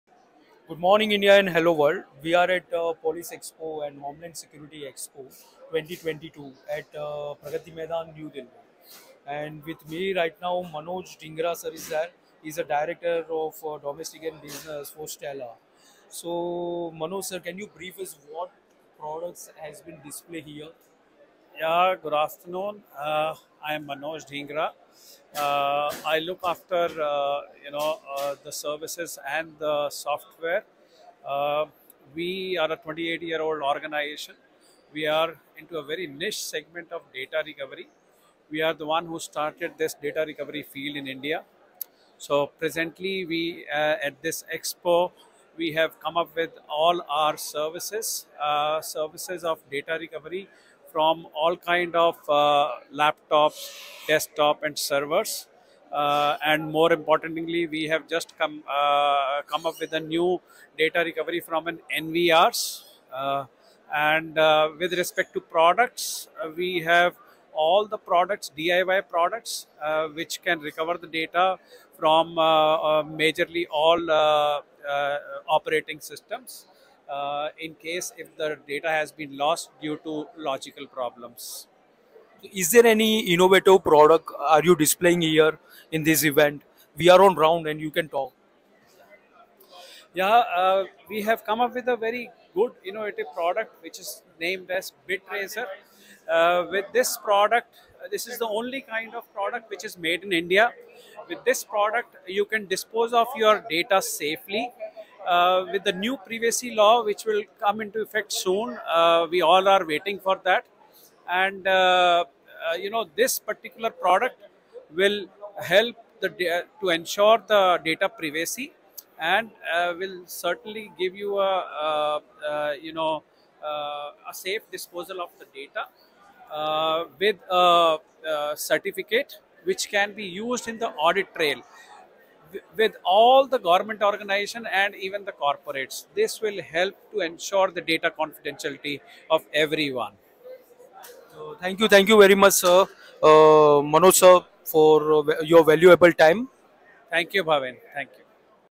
Mr. Manoj Dhingra-Director Domestic BusinessSTELLAR INFORMATION TECHNOLOGY PVT LTD at PoliceExpo2022
Mr. Manoj Dhingra - Director Domestic Business ,
STELLAR INFORMATION TECHNOLOGY PVT LTD
at Police Expo 2022 Pragati Maidan
Hall no 10 Stall no A56
coverage by INDUSTRIAL HEALTH & SAFETY REVIEW